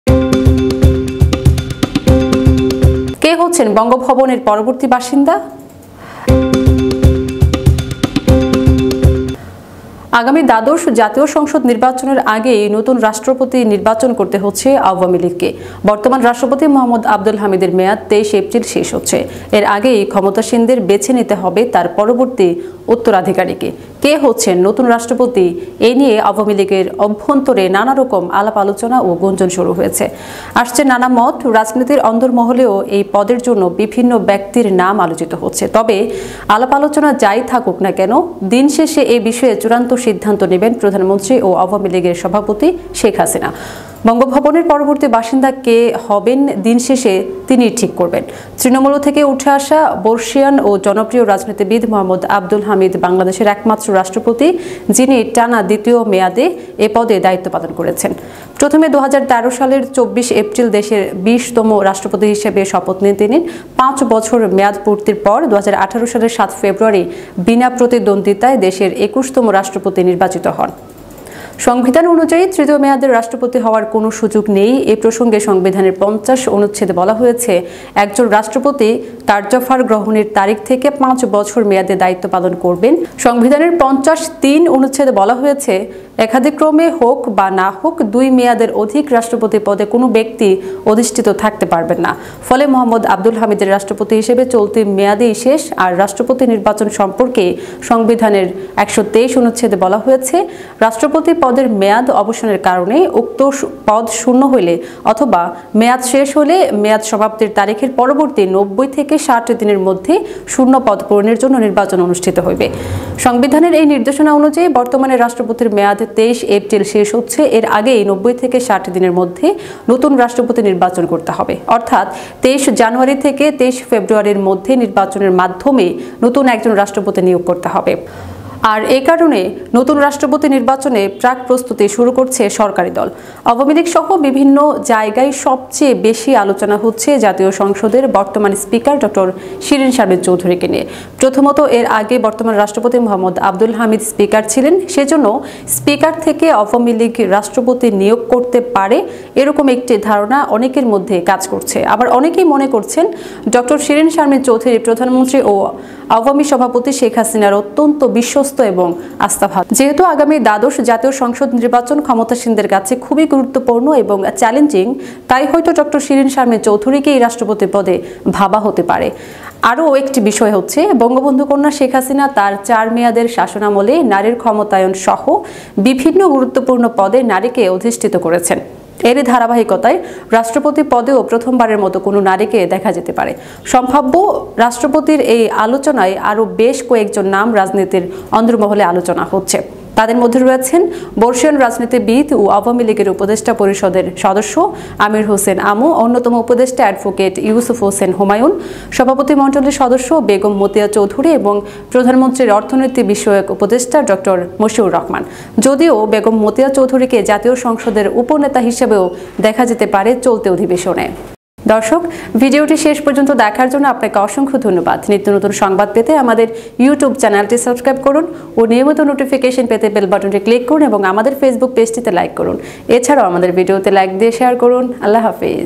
কে হচ্ছেন বাংলাদেশের পরবর্তী রাষ্ট্রপতি | Bangladesh President | Election | Latest Bangladeshi News
আগামী দ্বাদশ জাতীয় সংসদ নির্বাচনের আগেই নতুন রাষ্ট্রপতি নির্বাচন করতে হচ্ছে আওয়ামী লীগকে। বর্তমান রাষ্ট্রপতি মো. আবদুল হামিদের মেয়াদ ২৩ এপ্রিল শেষ হচ্ছে। এর আগেই ক্ষমতাসীনদের বেছে নিতে হবে তার পরবর্তী উত্তরাধিকারীকে। কে হচ্ছেন নতুন রাষ্ট্রপতি-এ নিয়ে আওয়ামী লীগের অভ্যন্তরে নানারকম আলাপ-আলোচনা ও গুঞ্জন শুরু হয়েছে। আসছে নানা মত। রাজনীতির অন্দরমহলেও এই পদের জন্য বিভিন্ন ব্যক্তির নাম আলোচিত হচ্ছে। তবে আলাপ-আলোচনা যাই থাকুক না কেন, দিন শেষে এ বিষয়ে চূড়ান্ত সিদ্ধান্ত নেবেন প্রধানমন্ত্রী ও আওয়ামী লীগ সভাপতি শেখ হাসিনা।

ধন্যবাদ..